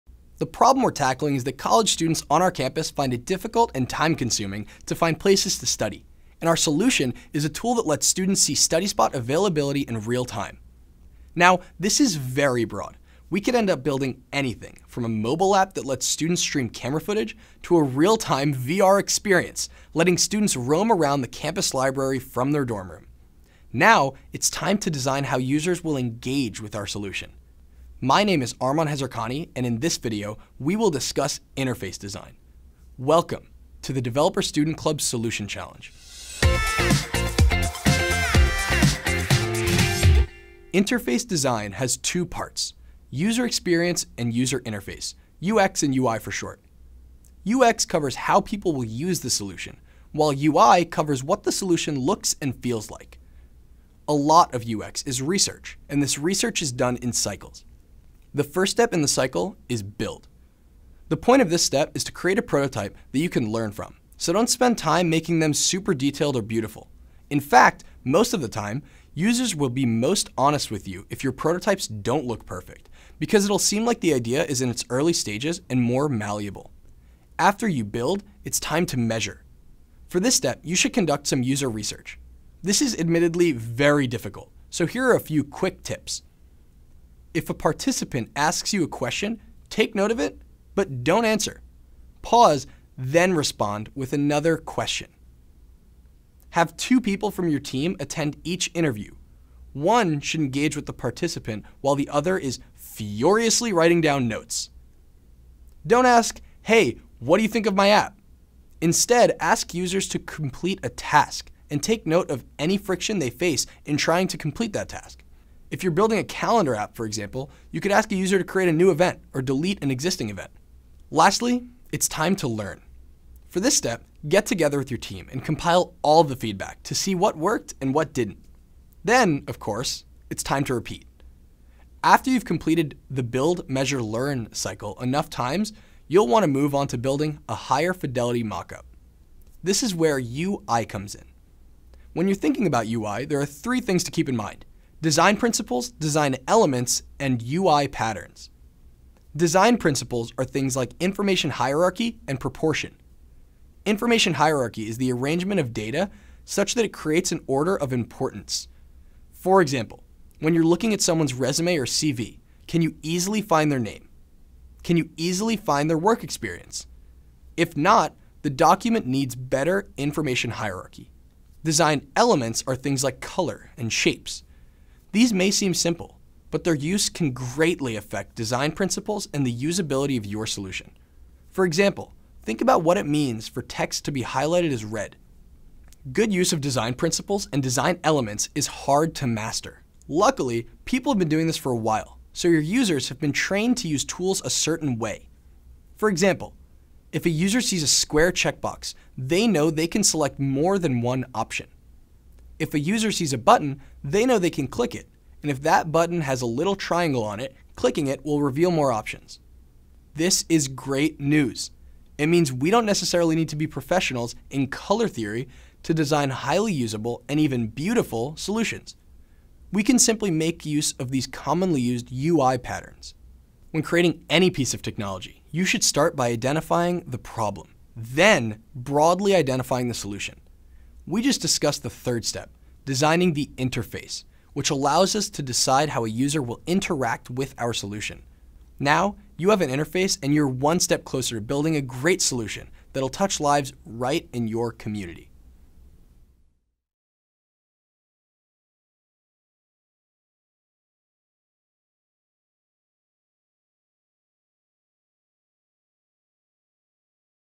DSC Solution Challenge: Designing the interface
Here at Developer Student Clubs, the point of technology is to use it to build real solutions to real problems in our community.

In our last video, we talked about identifying a solution. Now, let’s jump into the 2 parts of interface design how users will engage with our solution. Watch now and find out with our host Arman Hezarkhani!